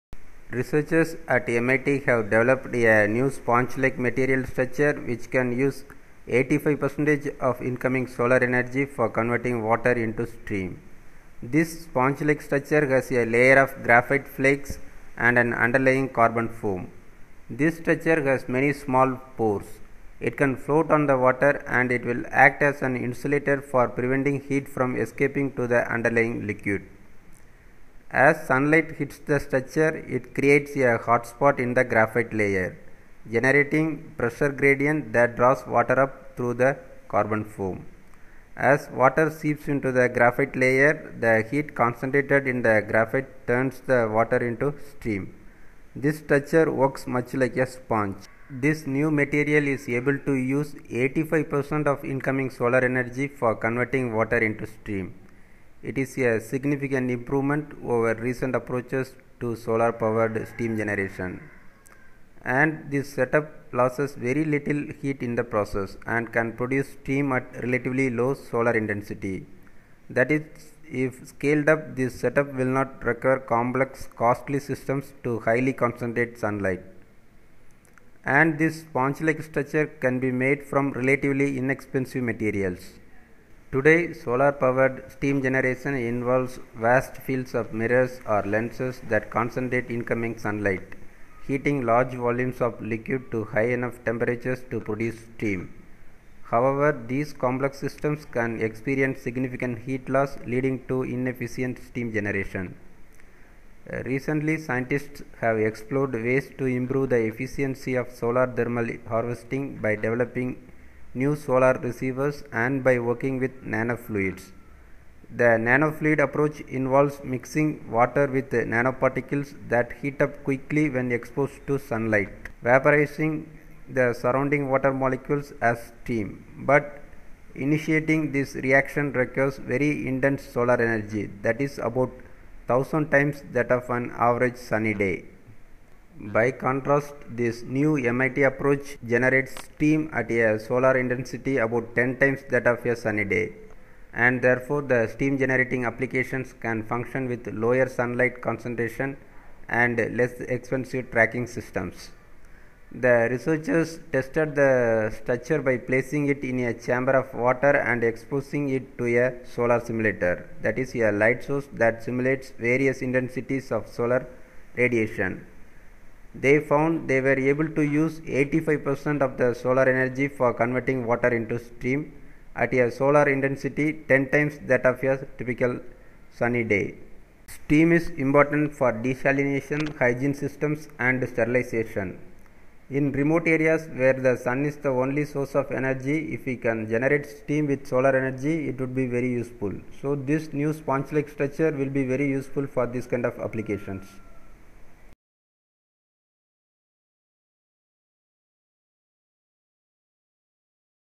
New spongelike material for effective Solar steam generation by heat localization
Researchers at MIT have developed a new spongelike material structure which can use 85% of incoming solar energy for converting water into steam.

This spongelike structure  has a layer of graphite flakes and an underlying carbon foam.  This structure has many small pores.
It can float  on the water, and it will act as an insulator for preventing heat from escaping to the underlying liquid, .
As sunlight hits the structure, it creates a hotspot in the graphite layer, generating a pressure gradient that draws water up through the carbon foam. As water seeps into the graphite layer, the heat concentrated in the graphite turns the water into steam. This structure works much like a sponge.

This new material is able to use 85 percent of incoming solar energy for converting water into steam. It is a significant improvement over recent approaches to solar-powered steam generation. And, this setup loses very little heat in the process, and can produce steam at relatively low solar intensity. i-e if scaled up, this setup will not require complex, costly systems to highly concentrate sunlight.

And, this spongelike structure can be made from relatively inexpensive materials.

Today, solar-powered steam generation involves vast fields of mirrors or lenses that concentrate incoming sunlight, heating large volumes of liquid to high enough temperatures to produce steam. However, these complex systems can experience significant heat loss, leading to inefficient steam generation.

Recently, scientists have explored ways to improve the efficiency of solar-thermal harvesting by developing new solar receivers and by working with nanofluids. The nanofluid approach involves mixing water with nanoparticles that heat up quickly when exposed to sunlight, vaporizing the surrounding water molecules as steam. But initiating this reaction requires very intense solar energy, i-e about 1,000 times that of an average sunny day.

By contrast, this new MIT approach generates steam at a solar intensity about 10 times that of a sunny day . And, therefore the steam-generating applications can function with lower sunlight concentration and less-expensive tracking systems.

The researchers tested the structure by placing it in a chamber of water and exposing it to a solar simulator, i-e a light source that simulates various intensities of solar radiation. They found they were able to use 85 percent of solar energy for converting water into steam at a solar intensity 10 times that of a typical sunny day.

Steam is important for desalination, hygiene systems, and sterilization.

In remote areas where the sun is the only source of energy, if we can generate steam with solar energy, it would be very useful. So, this new spongelike structure will be very useful for these kind of applications.